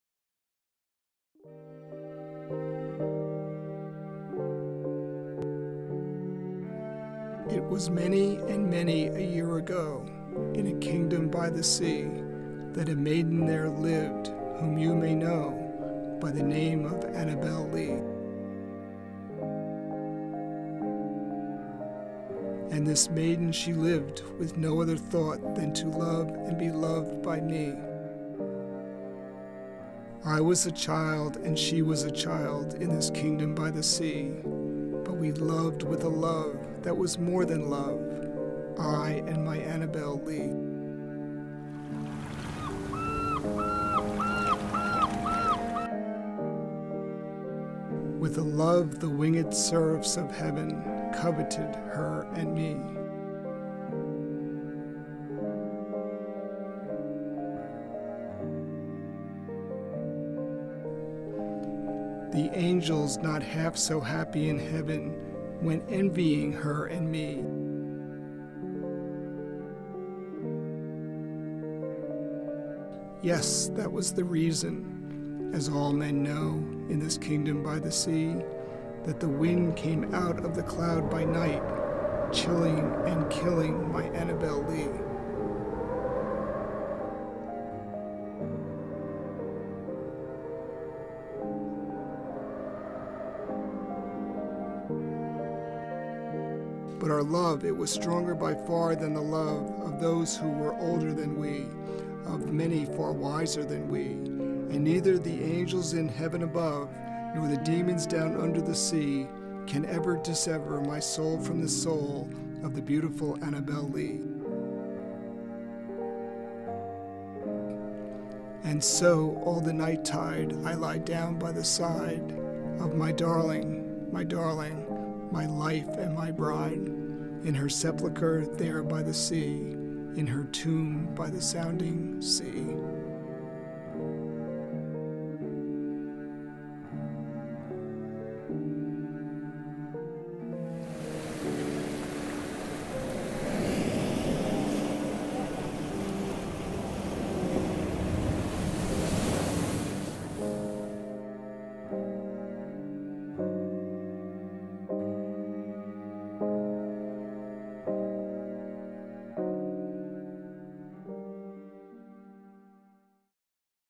Annabel Lee Short Movie Project
This video was made for a video class that I took in 2016. The story is based on the poem "Annabel Lee" by Alan Edgar Poe. Hope you guys enjoyed it!